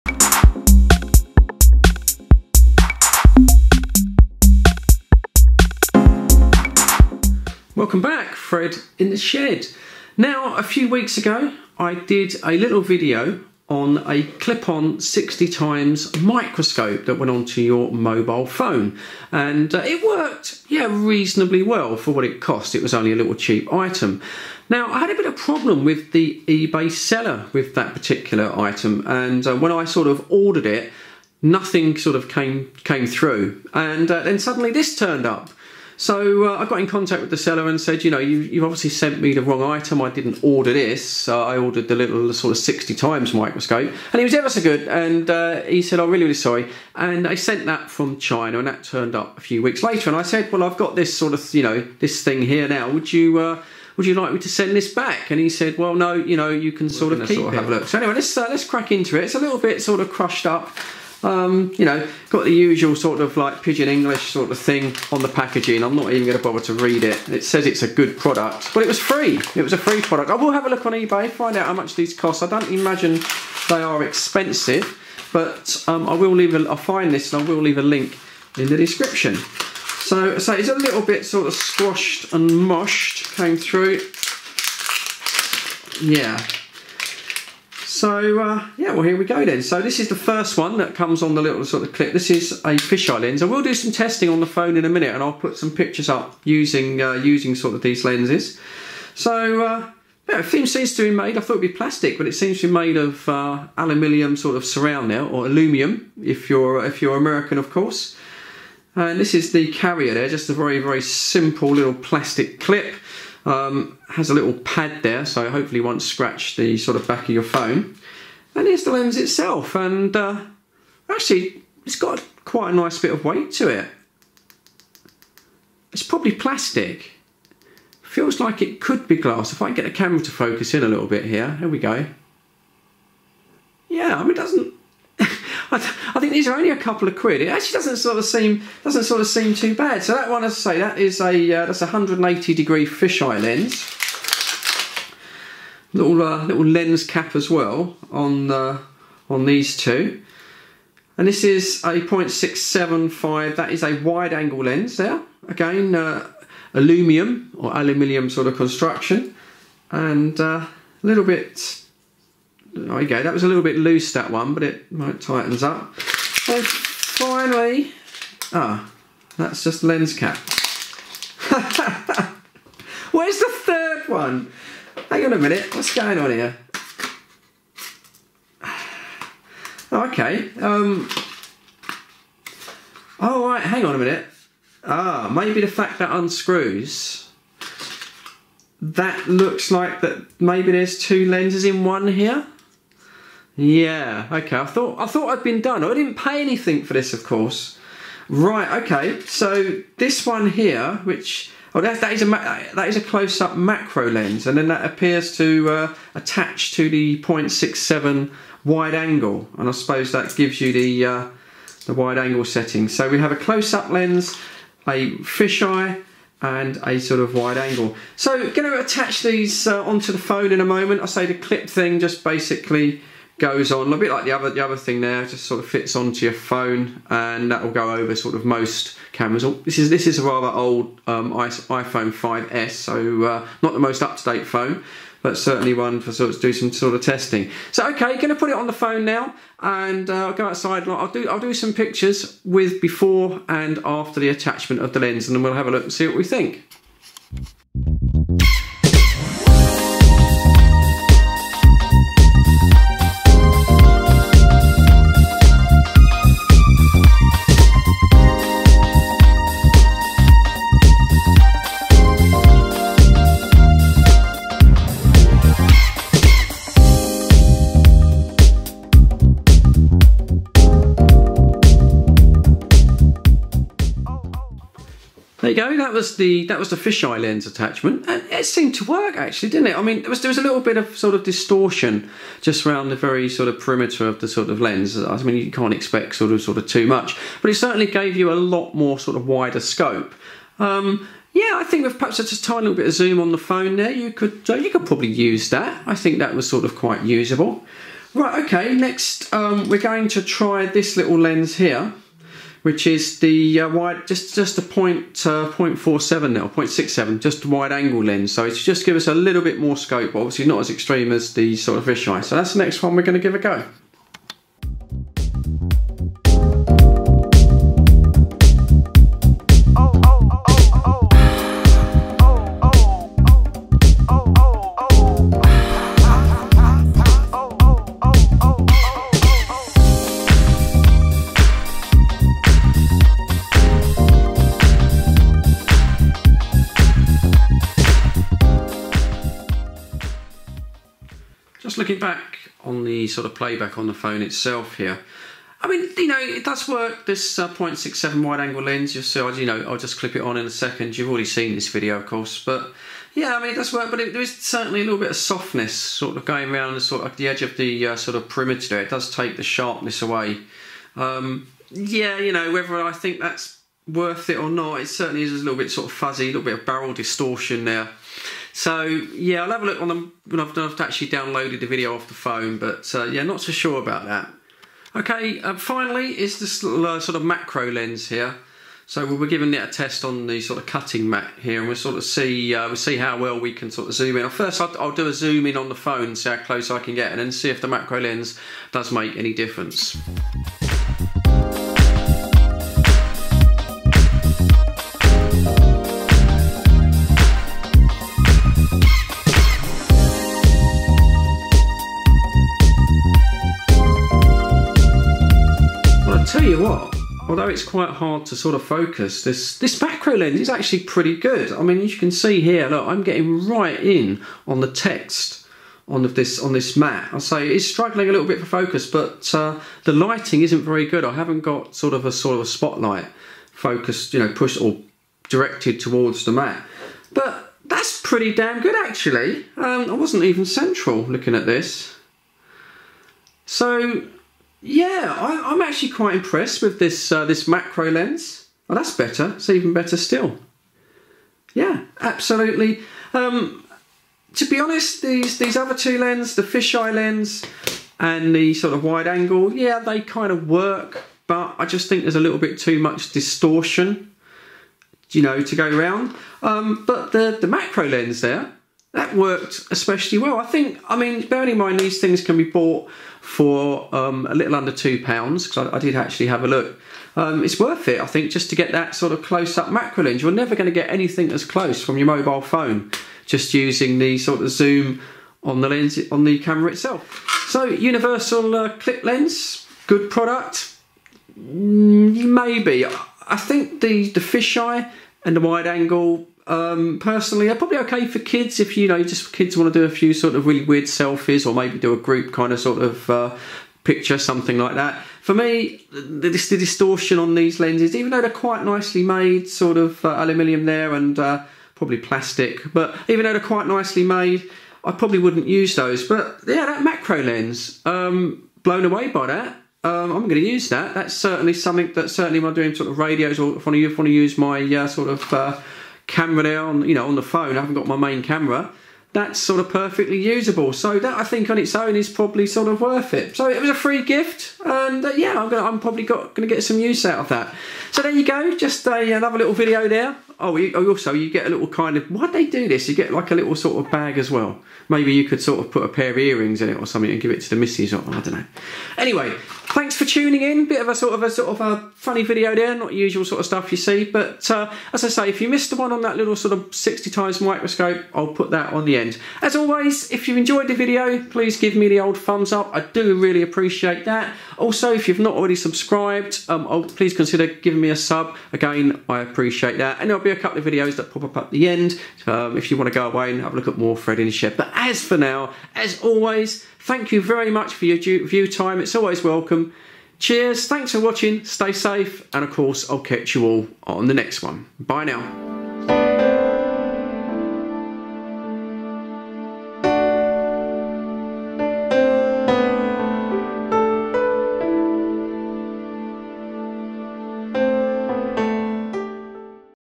Universal 3 in 1 Clip On smart mobile phone Lens set.
Cheap Ebay 3 in 1 Fish Eye Wide Angle Macro Clip On Camera Lens Set for Iphone smart Mobile Phone Tablet. Fits iphone android and galaxy devices.